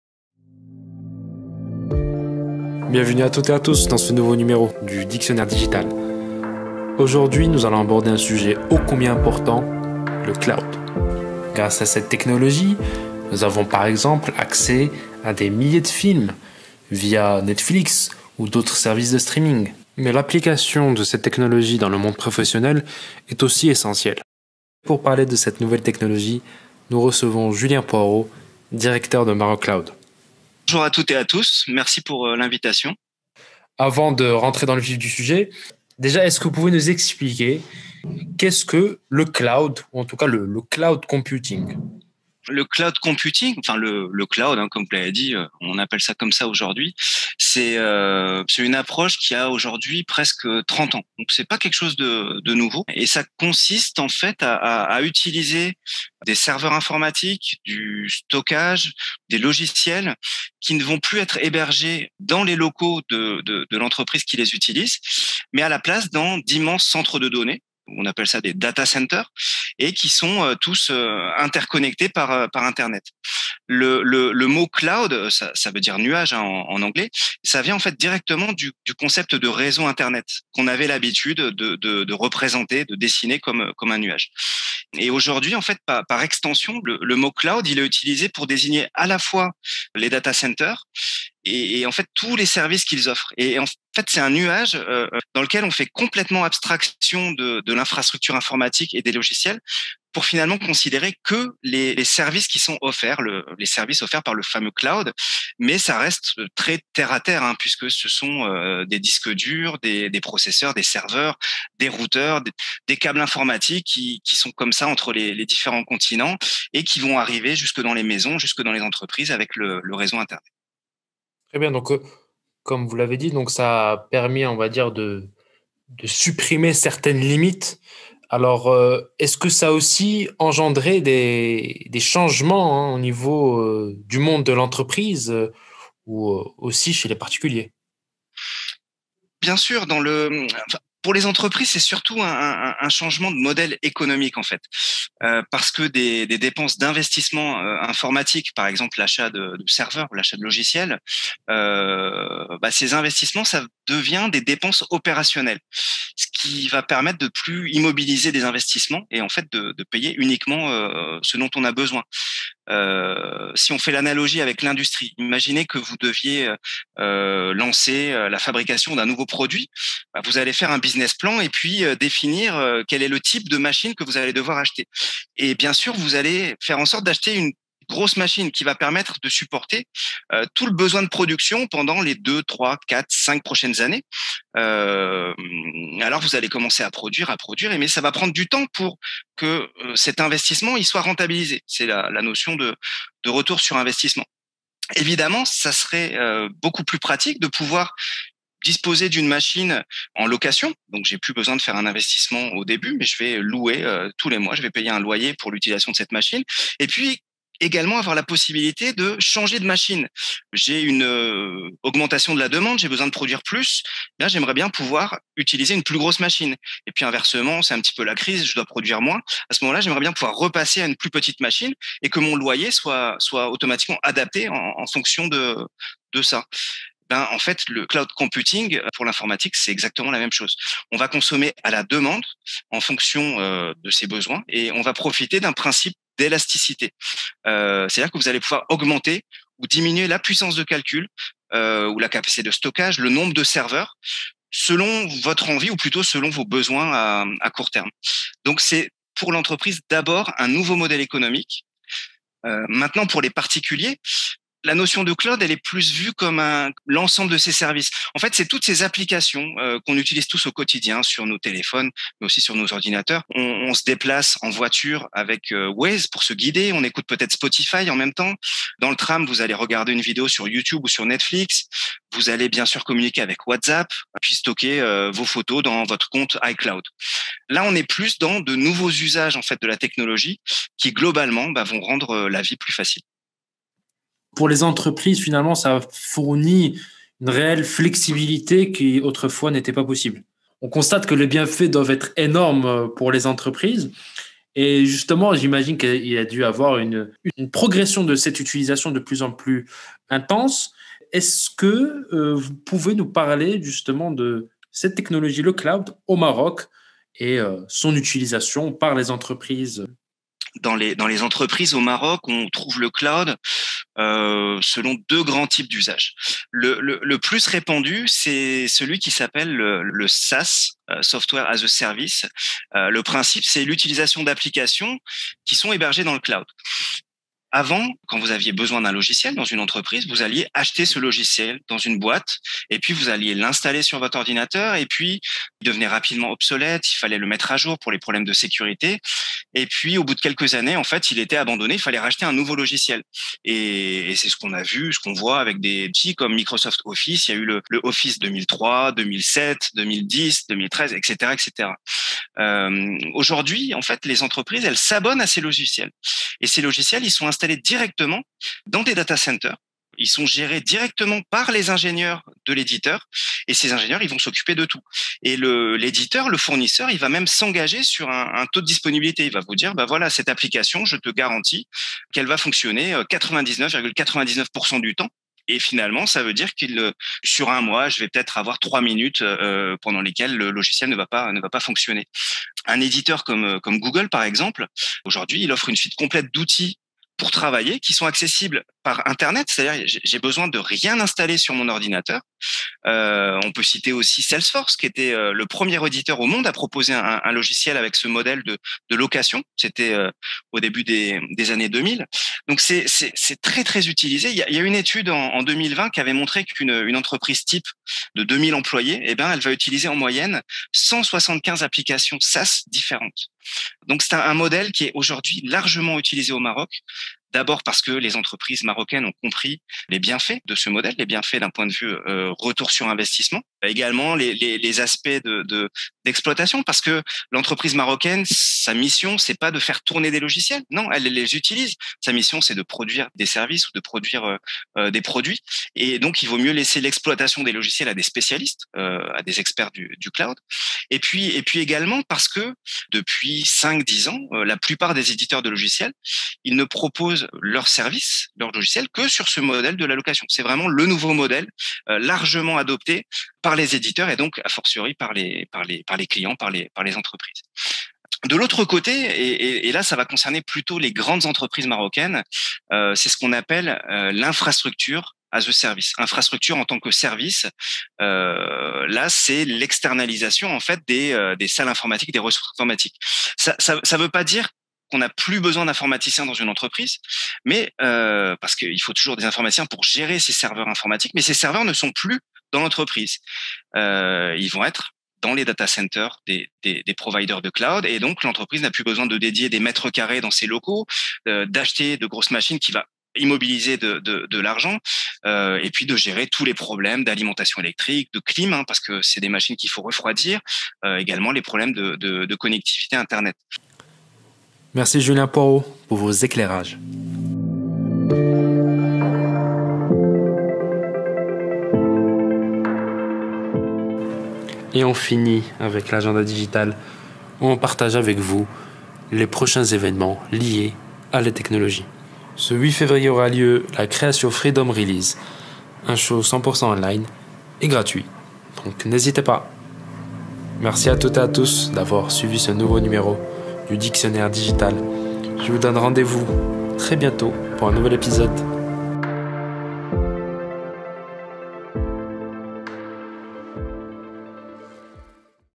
Dictionnaire digital : Les enjeux du Cloud
Retour du dictionnaire digital, avec cette fois une thématique à la fois complexe et essentielle dans le paysage technologique d'aujourd'hui : le Cloud.